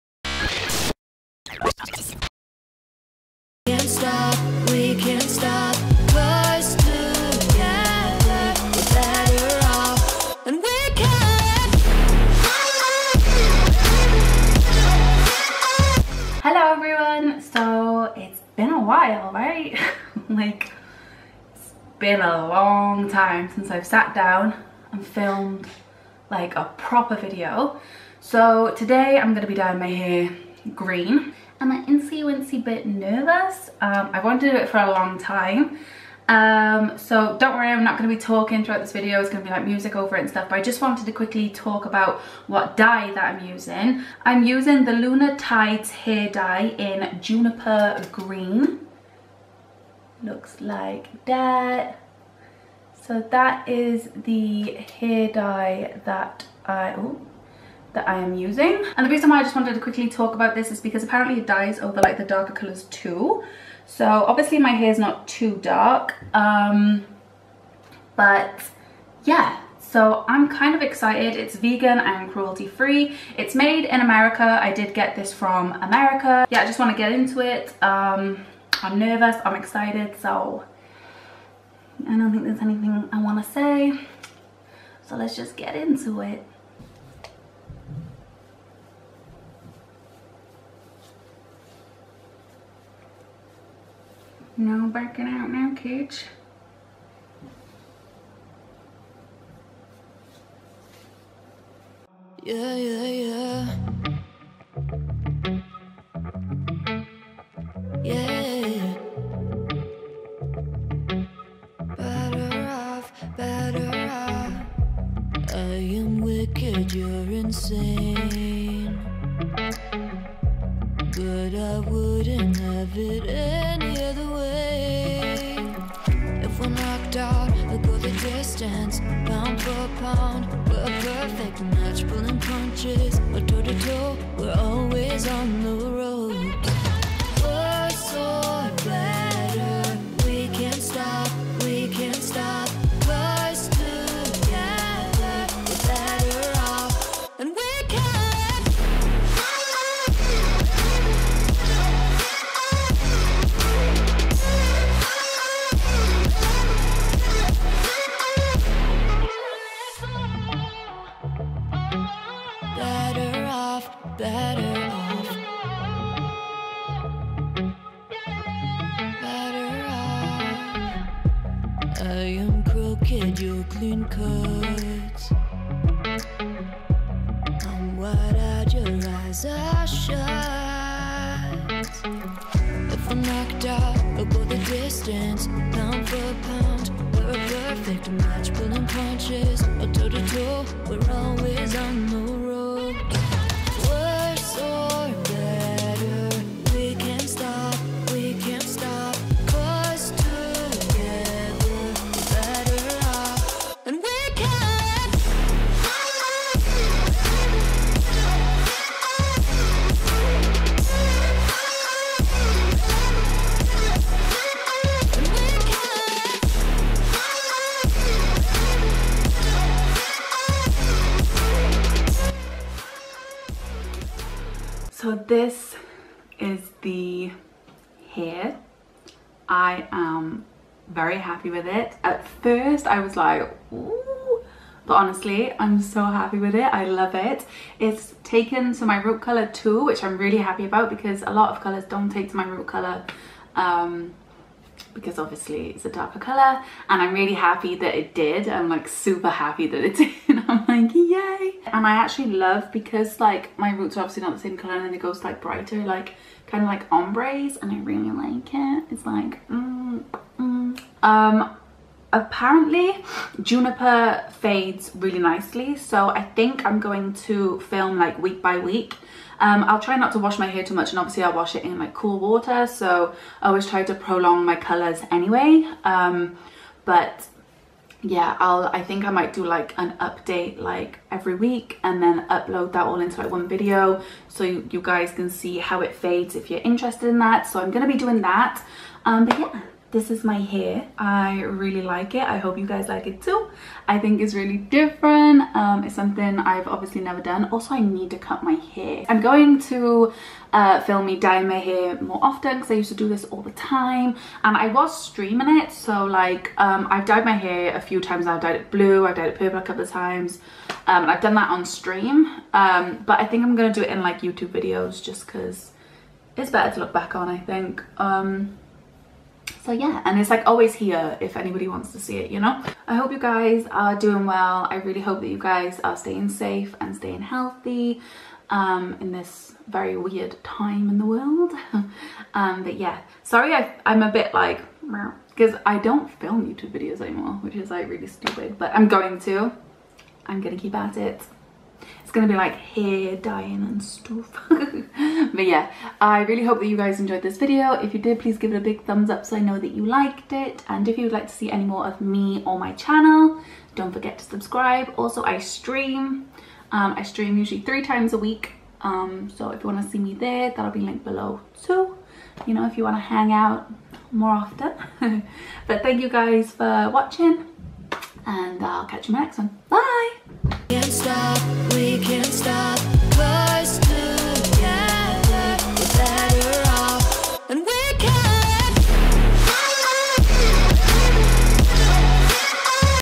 finally dying my hair green | hair transformation
it's been a while since i uploaded a video of me dying my hair! decided to finally dye my hair green, after wanting to do it for a loooong time! i hope you enjoy the video! xxx